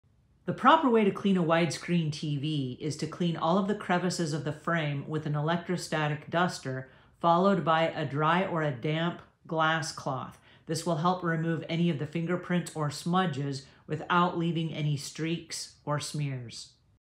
The Proper Way to Clean a Wide-Screen TV - Pro Cleaning Tip
The proper way to clean a wide-screen TV comes down to having the proper tools. It's recommended to use an electrostatic duster to remove the dust from the crevices of the frame. (Shown is a Home portable duster with case - paid followed by a dry or damp cleaning glass cloth (shown & paid this will remove any smudges and fingerprints without leaving any streaking.

Electrostatic Feather Duster
LED HDTV
Microfiber Cleaning Cloths
Bib Apron - Unisex Black Aprons
Powder-Free Nitrile Exam Gloves